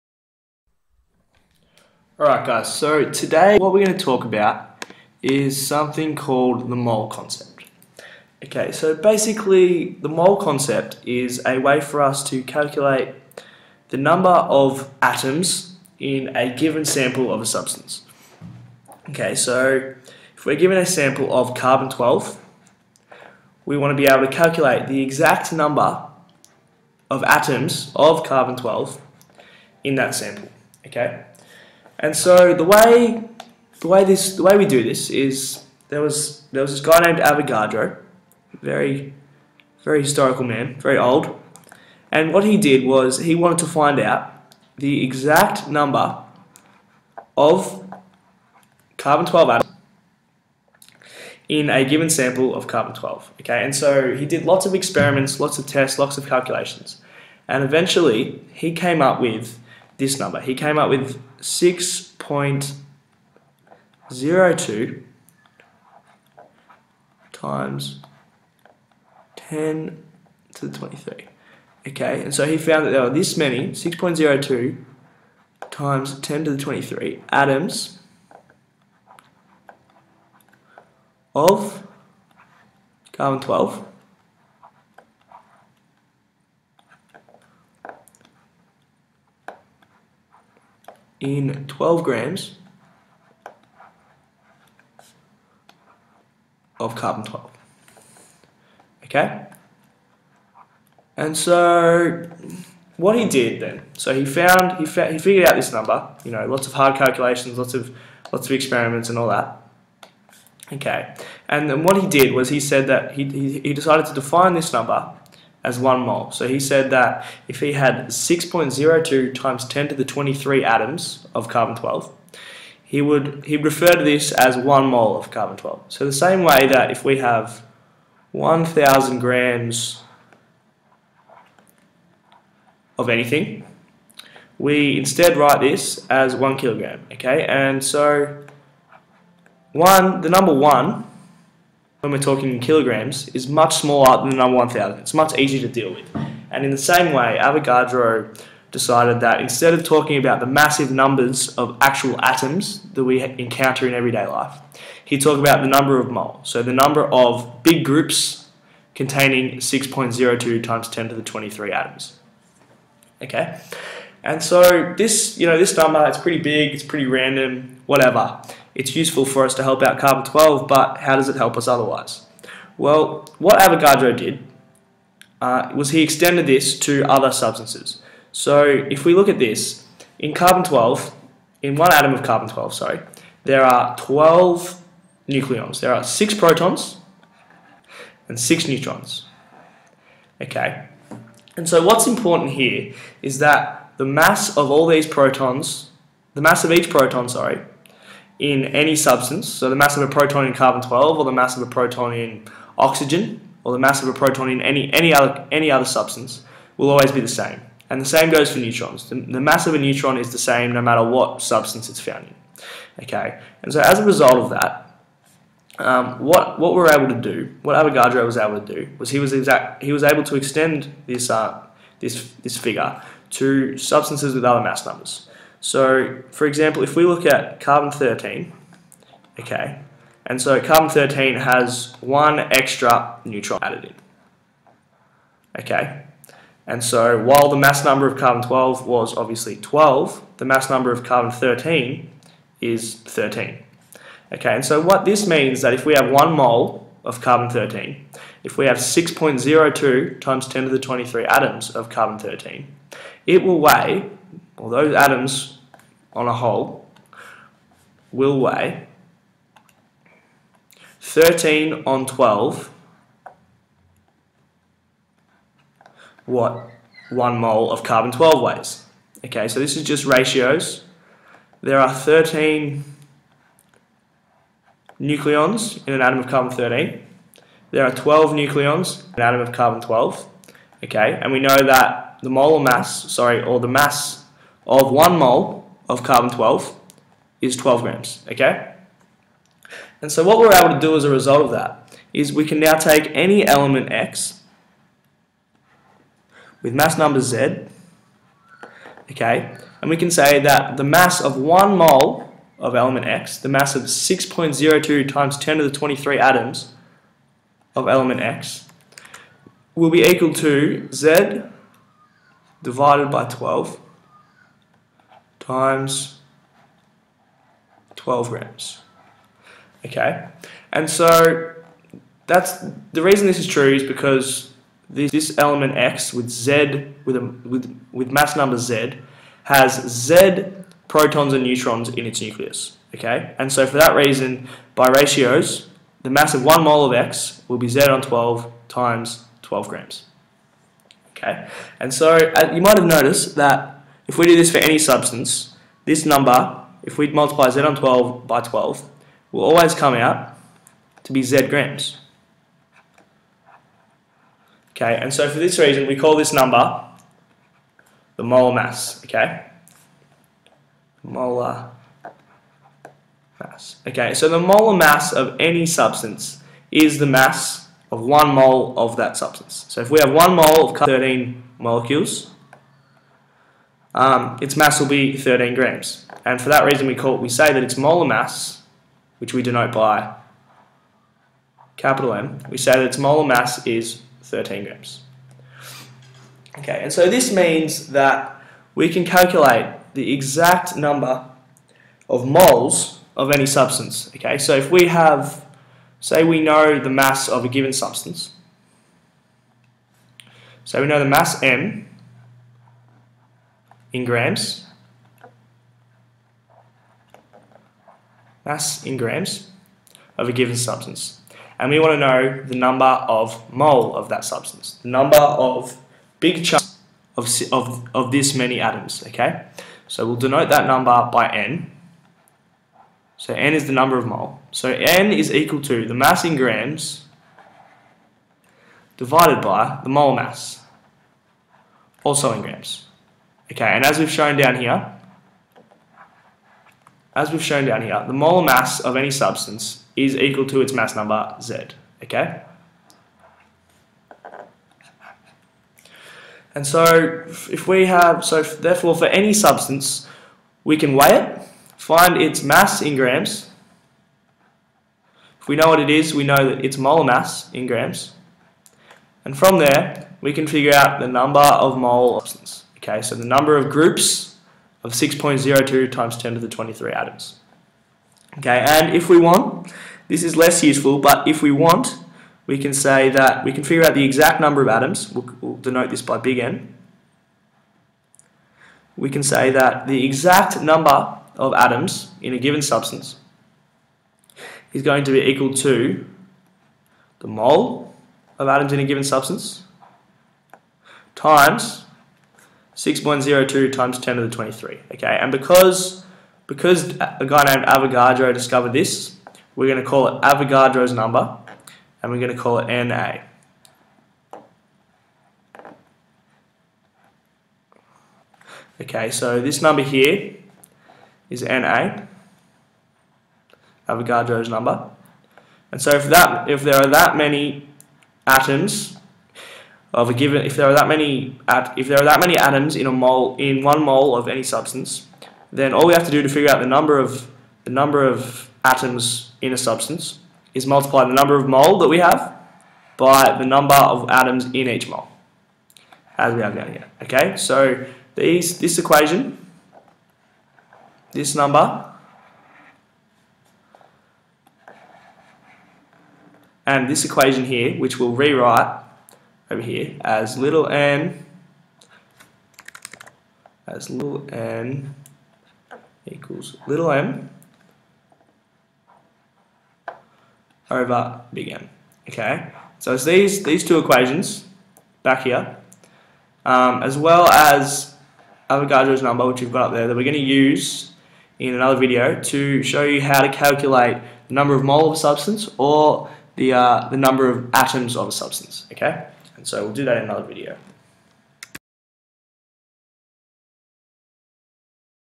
Mole Concept
In this video, we look at the idea of the 'mole' and introduces Avodgadro's number. I show how we can begin to use these ideas to make chemical analysis much easier by calculating quantities of substances.